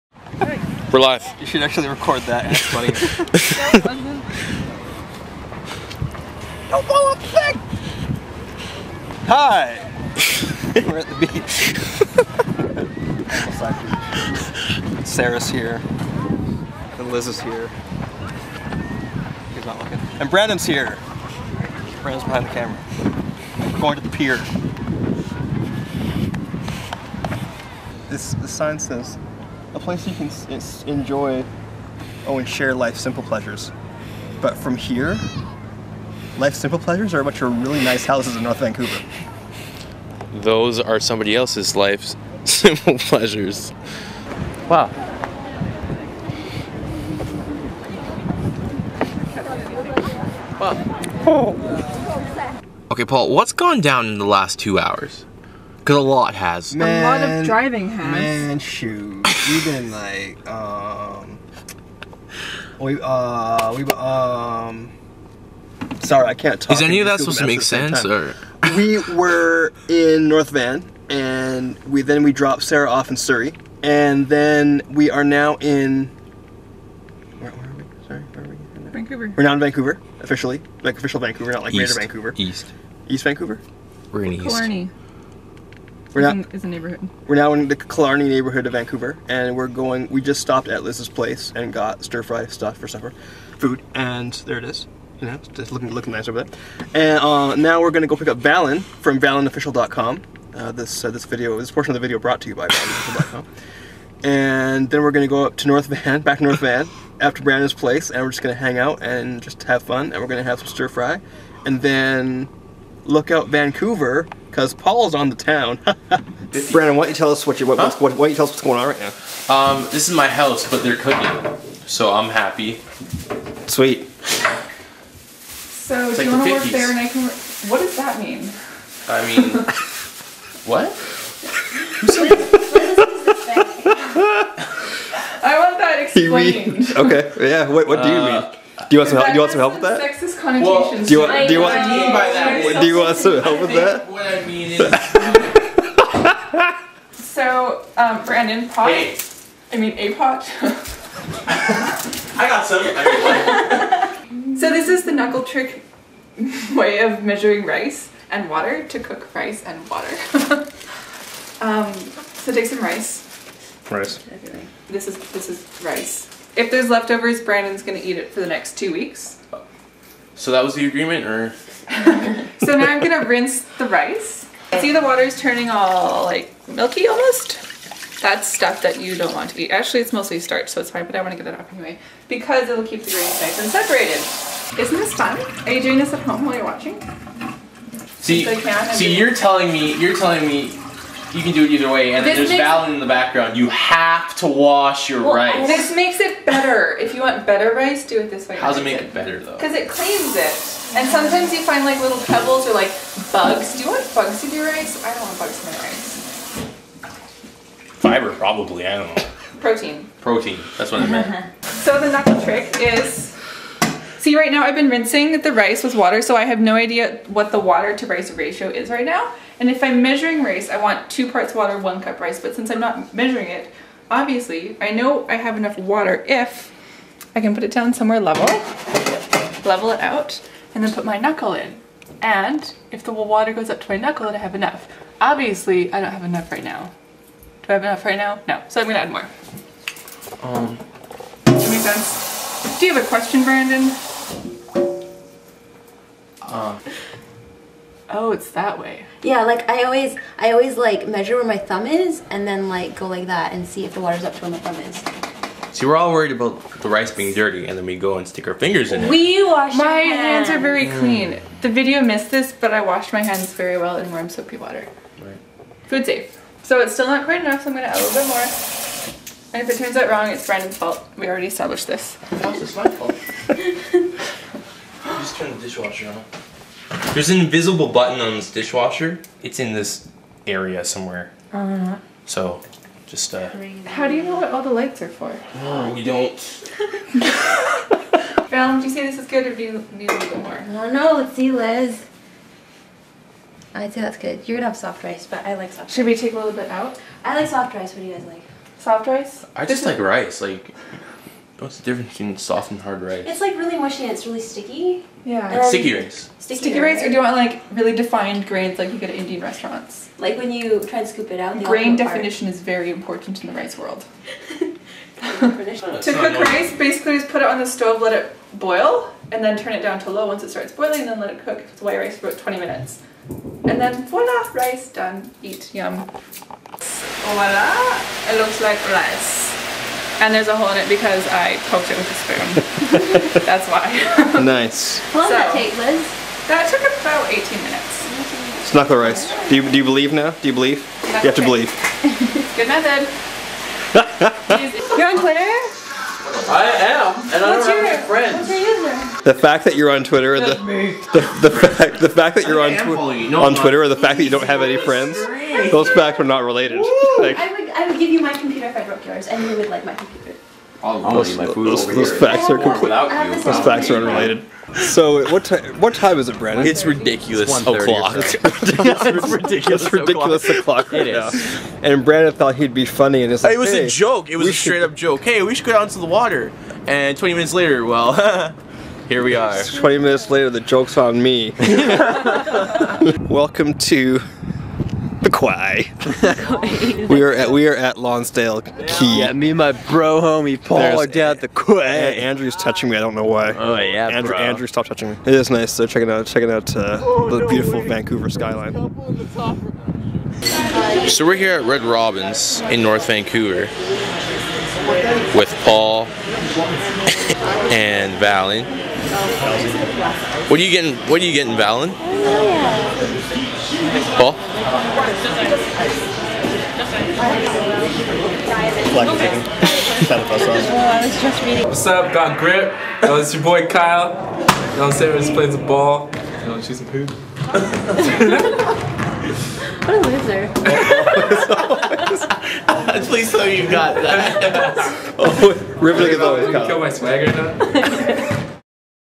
Easter '11
Clips from my trip to Vancouver over Easter '11.